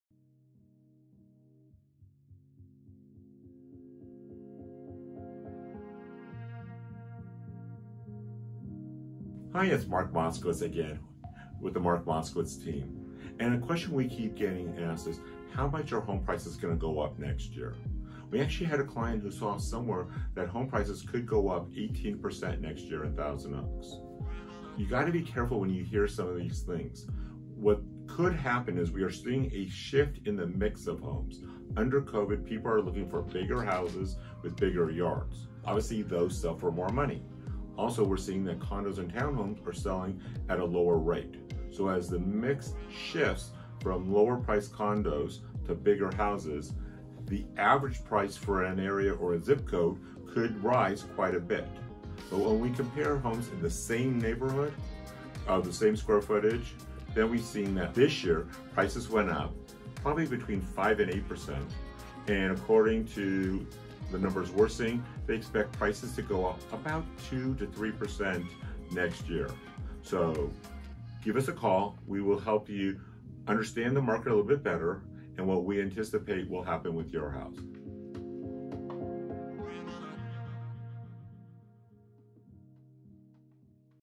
Are Prices Going Up?
In today's episode Mark dives into the projected price movement for 2021. Will they raise as much as they did in 2020? Come find out... Don't miss any episodes and keep learning about the home selling process with The Mark Moskowitz Team.